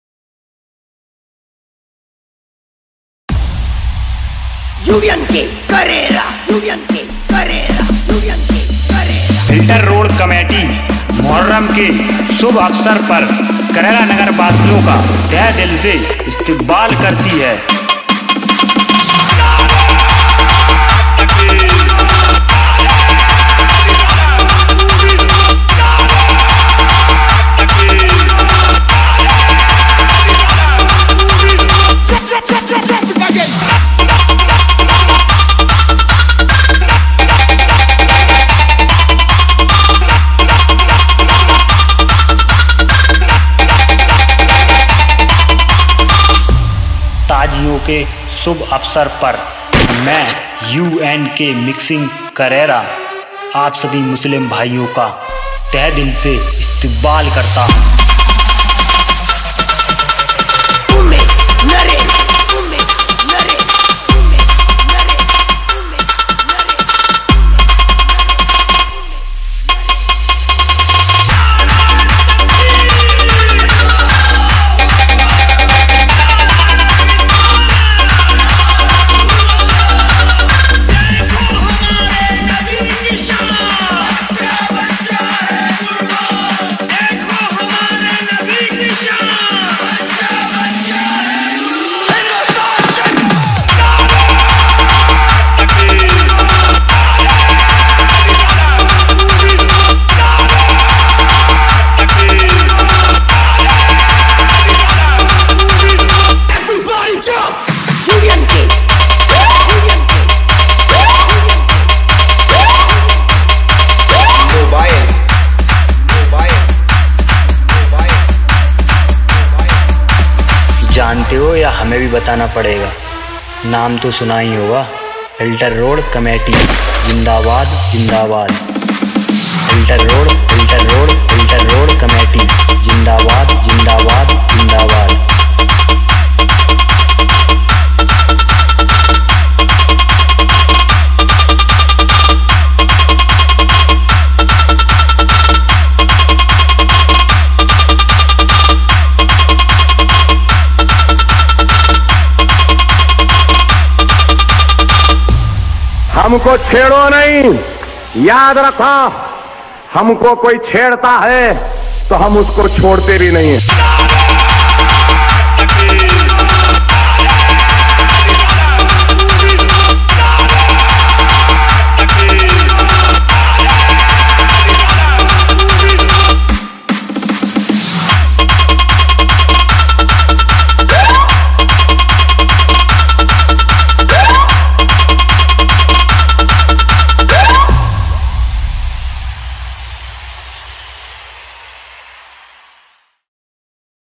NEW MUHHARAM🇵🇰 SPECIAL 2023💥💥 EDM MIX FILTER ROAD CAMETY KARERA DJ AADILKARERA🔥djremix
new muhharaam nara intro🇵🇰🇵🇰💥💥.
.filter road COMMETY 🇵🇰⚡KARERA 🔥🔥
new nara edm mix .hard bass 🔥🔥
.    plz watch full song 🙃🙃🔥🔥

credit= UNK MIXING KARERA_&_DJ AADIL KARERA 🔥🔥
new muhharaam song remix.